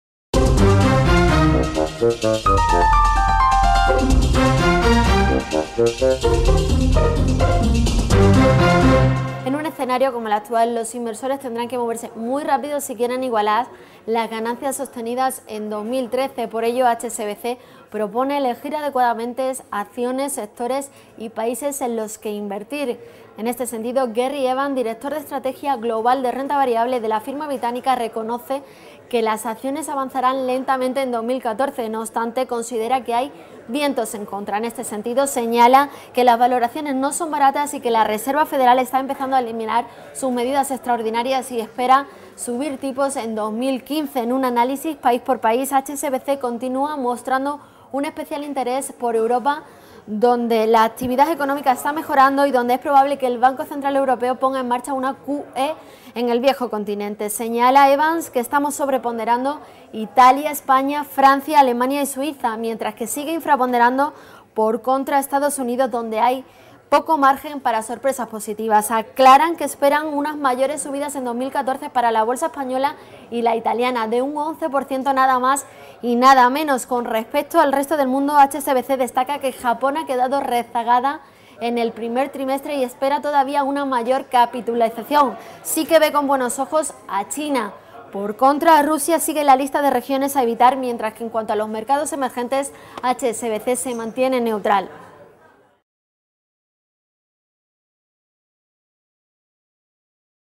Cómo hacer dinero en un mundo tan 'duro'
HSBC propone para superar el escenario actual elegir adecuadamente títulos, sectores y países. Las bolsas avanzarán en 2014 "lentamente", pero hay "vientos en contra".